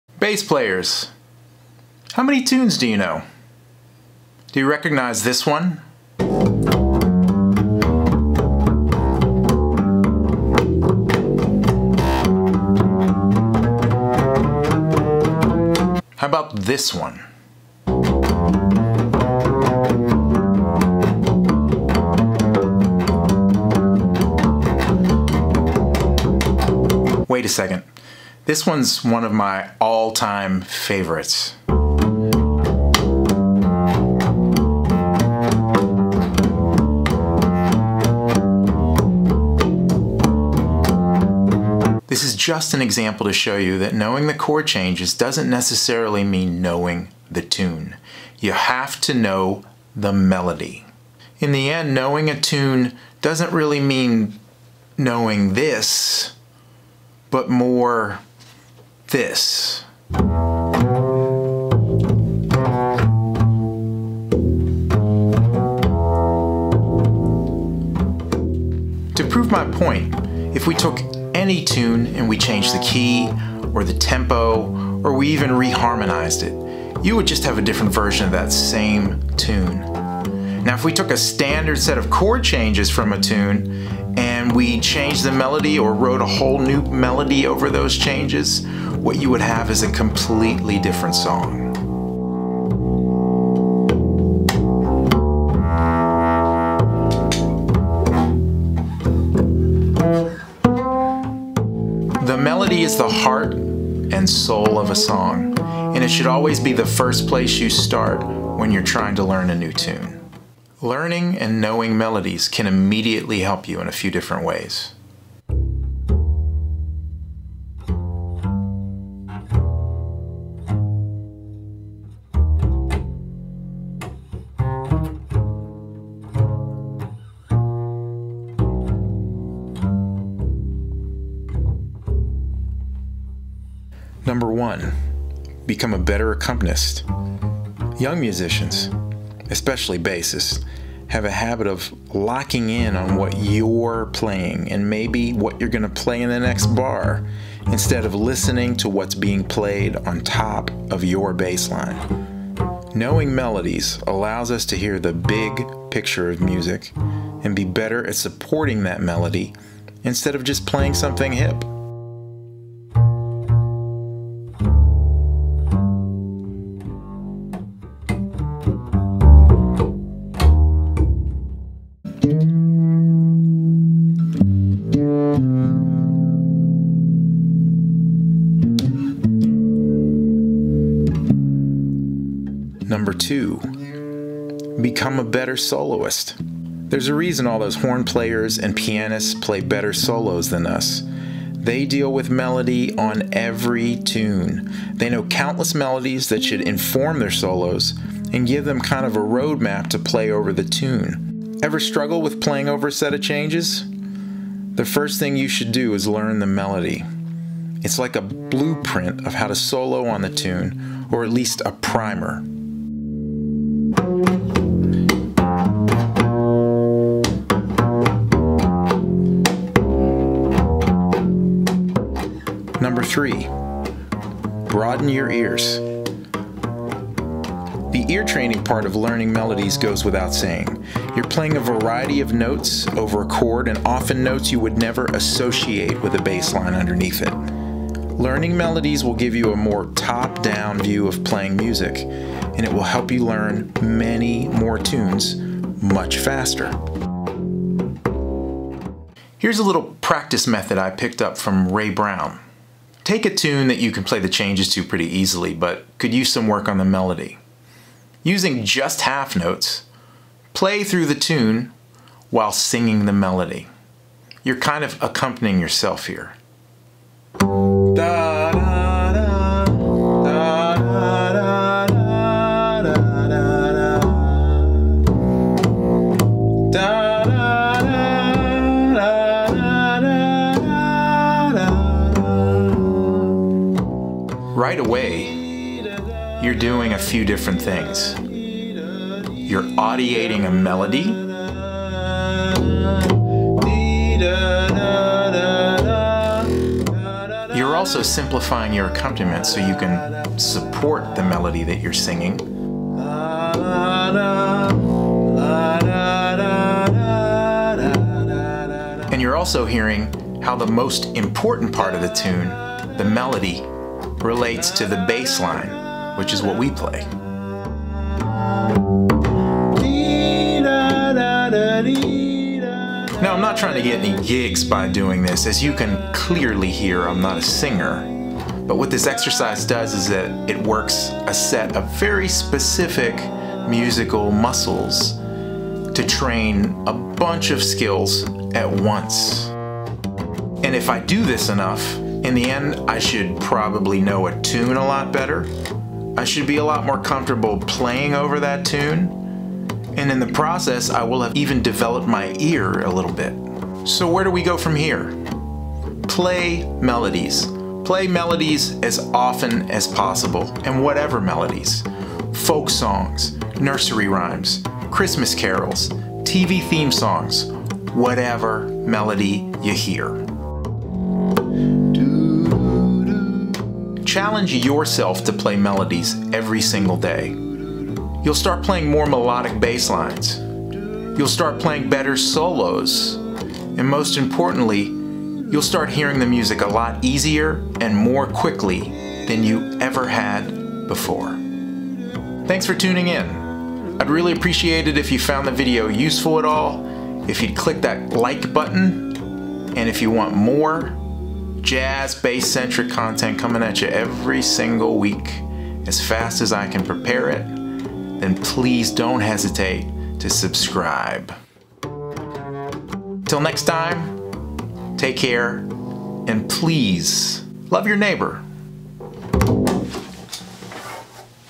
Jazz Lesson #9: Play the Melody!!
This week's video is a topic central to playing good music: MELODY.  It's not just for horn players!  Why is melody so important to us bassists and why would learning a melody (or 10) help us become better at playing bass?!  With a little help from some of my unused clips from earlier this year, along with a dash of bad singing, join me on this journey from the bottom of the music to the TOP.